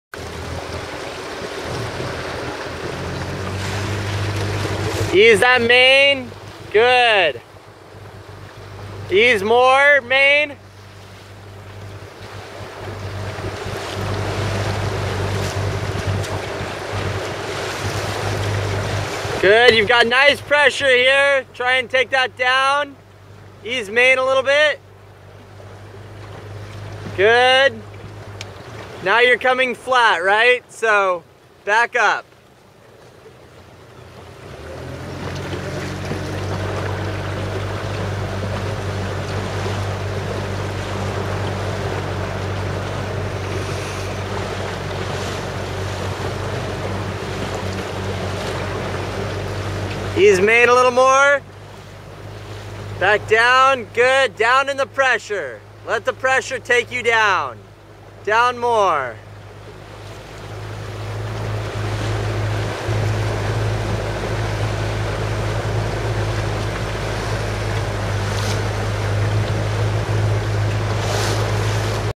Ian and Taylor Downwind (sideforce) - 5/17/2013
In general, I think that your downwind speed problems were a combination of too much weight out, forcing you to sail high to keep the boat from heeling to windward, and too little adjustment in changing conditions.  In this light wind it is very important to be super sensitive to tiny changes in the pressure, and respond accordingly with mainsheet trim, weight, and steering to keep the sails evenly pressured.  After this video you guys sat inside of the boat more, and seemed to be much more on pace.